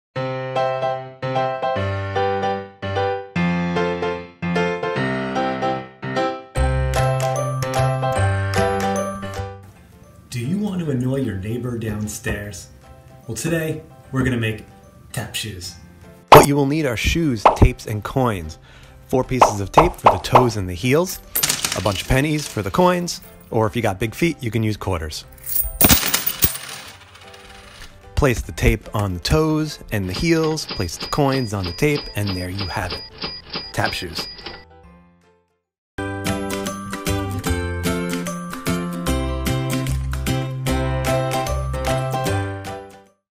Homemade with Education Through Music - Tap Shoes
ETM Music Teacher Mr. Gallagher walks you through making your own tap shoes! All you need is some shoes, change, and tape!  Perfect for students in grades 1-3.